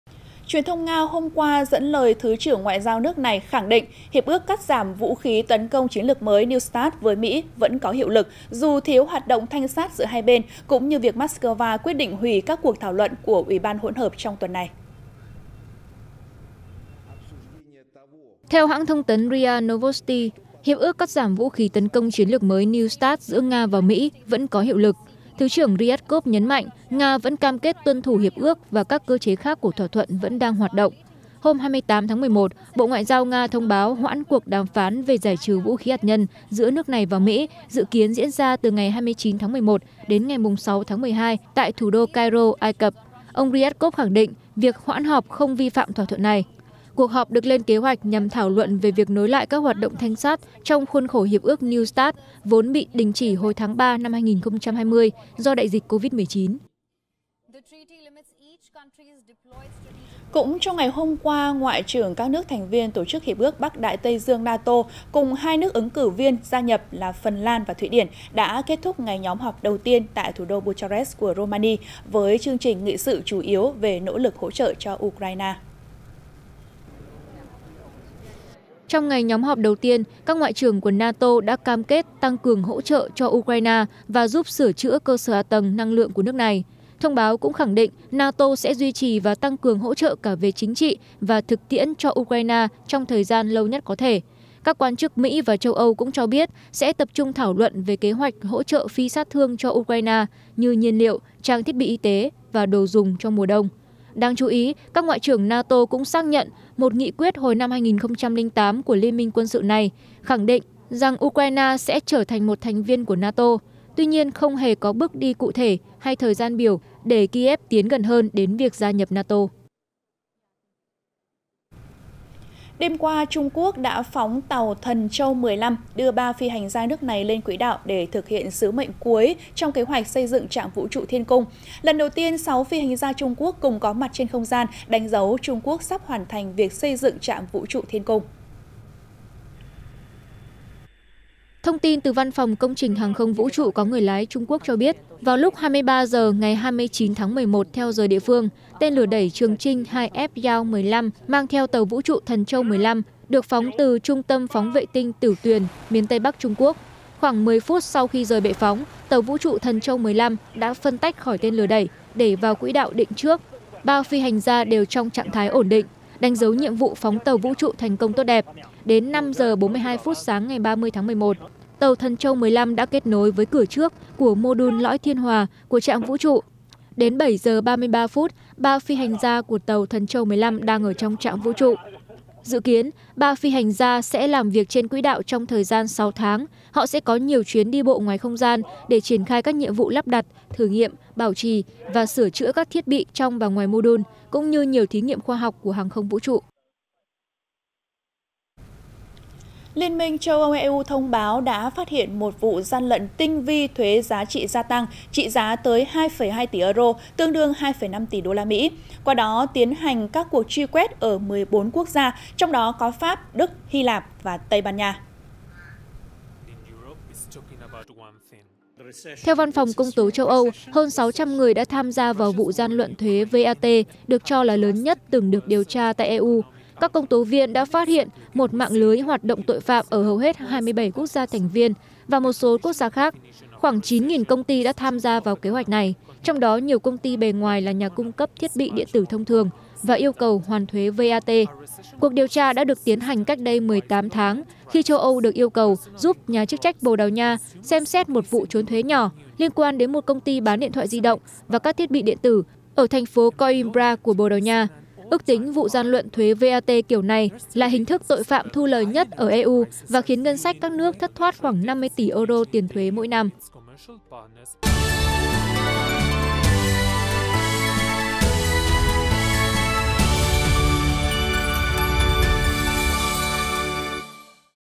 Nga khẳng định Hiệp ước New START vẫn còn hiệu lực | Tin quốc tế chiều 30/11
Nga khẳng định Hiệp ước New START vẫn còn hiệu lực | Tin quốc tế chiều 30/11 | Truyền Hình Nhân Dân
1. Dự Báo Thời Tiết Hôm Nay